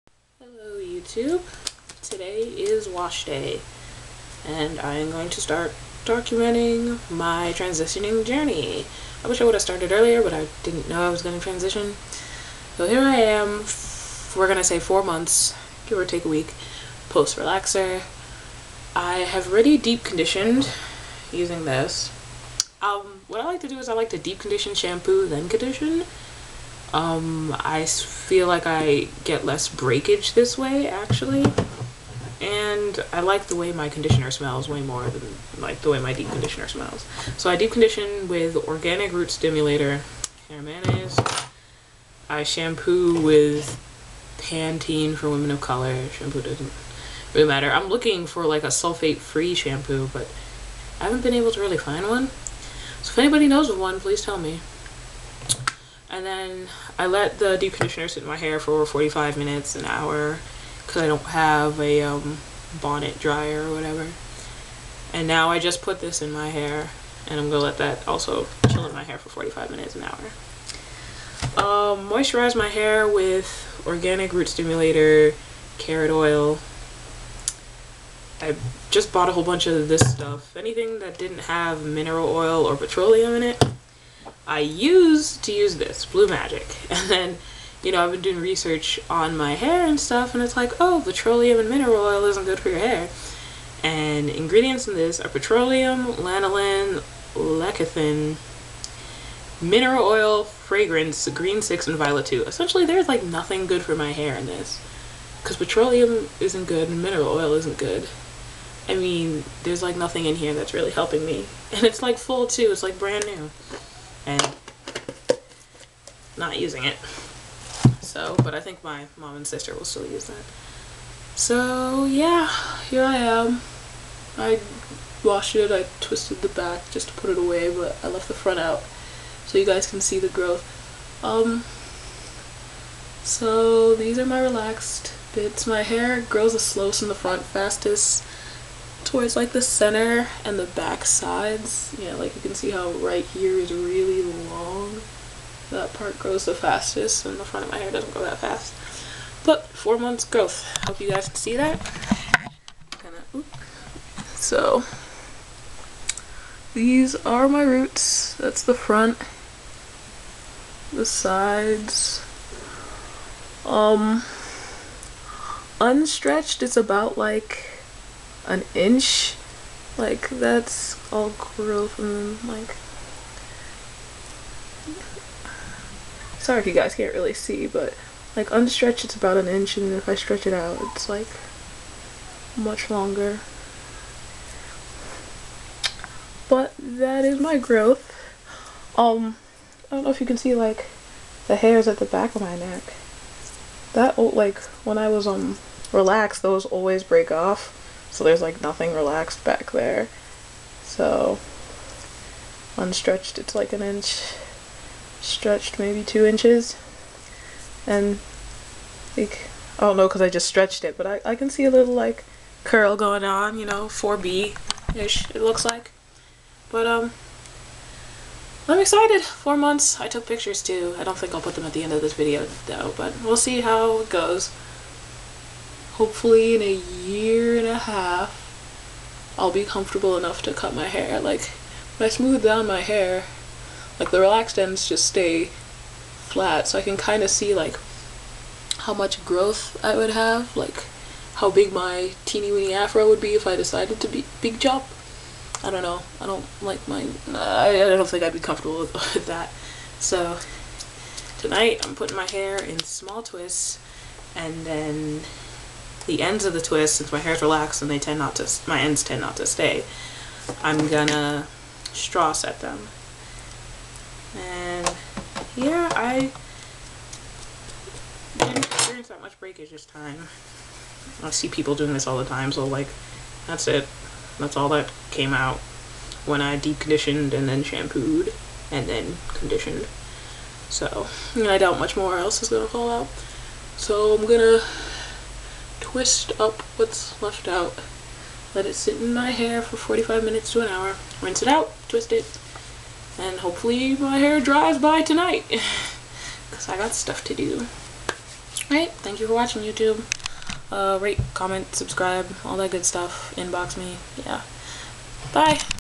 4 Month Transition - Wash Day
My head looks really big when I show you guys my roots, I know. Anyway here is the start of me documenting my journey. I will try and do a length check every 1-2 months from here on out.